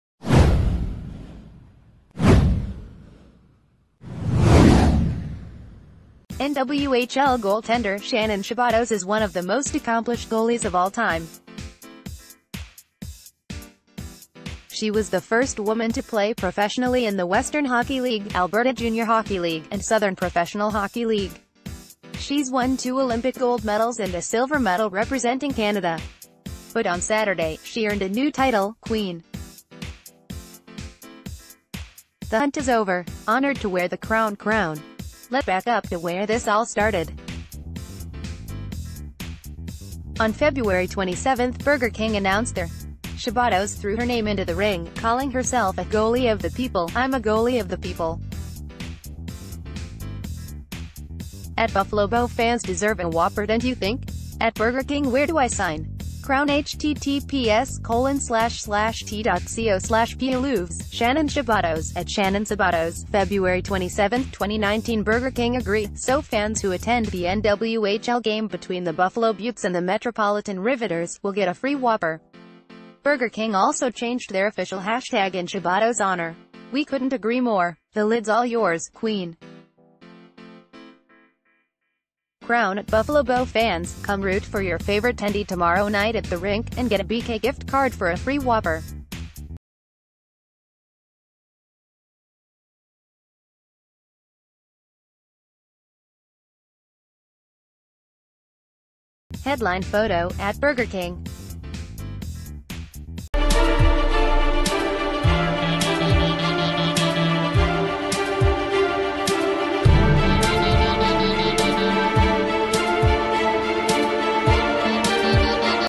NWHL goaltender volunteers to wear mask featuring Burger King’s face so fans get free Whoppers
NWHL goaltender Shannon Szabados is one of the most accomplished goalies of all time. She was the first woman to play professionally in the Western Hockey League, Alberta Junior Hockey League, and Southern Professional Hockey League. She’s won two Olympic gold medals and a silver medal representing Canada. But on Saturday, she earned a new title: Queen. The hunt is over! Honored to wear the crown 👑QueenAGoalie — Shannon Szabados (@ShannonSzabados) March 3, 2019 Durin...